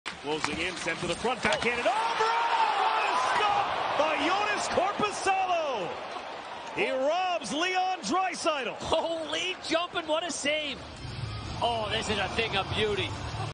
Save of the year candidate? 🤯
Joonas Korpisalo robs Leon Draisaitl with a fantastic glove save.

Feeling social?